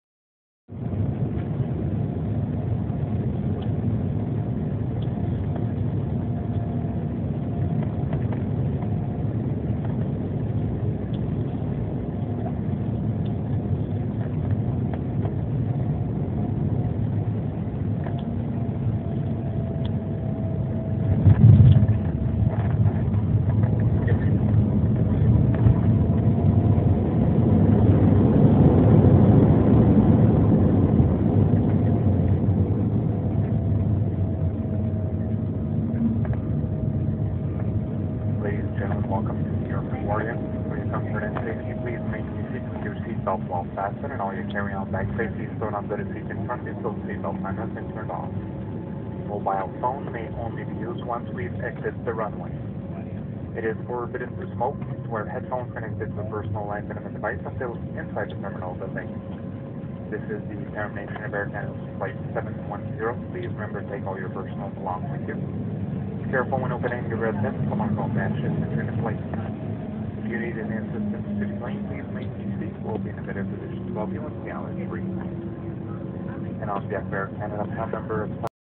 Landing at Laguardia Airport NYC
Landing at Laguardia Airport recorded from my cell phone...again, nothing special.